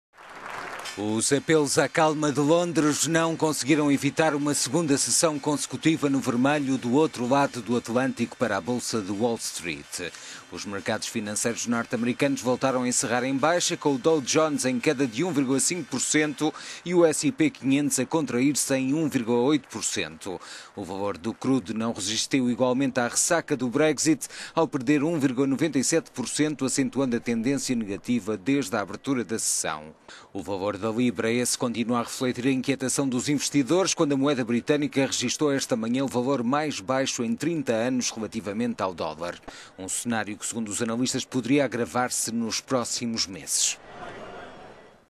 Ventos do 'Brexit' resfriam bolsa de Wall Street pelo segundo dia consecutivo
Os apelos à calma de Londres não conseguiram evitar uma segunda sessão consecutiva no vermelho, do outro lado do Atlântico, para a bolsa de Wall Street.

Os mercados financeiros norte-americanos voltaram a encerrar em baixa, com o Dow Jones Industrial em queda de 1,5% e o S&P500 a contrair-se em 1,8%.

O valor do crude não resistiu igualmente à ressaca do 'Brexit' ao perder 1,97%, acentuando a tendência negativa desde a abertura da sessão.

O valor da libra, esse, continua a refletir a inqui…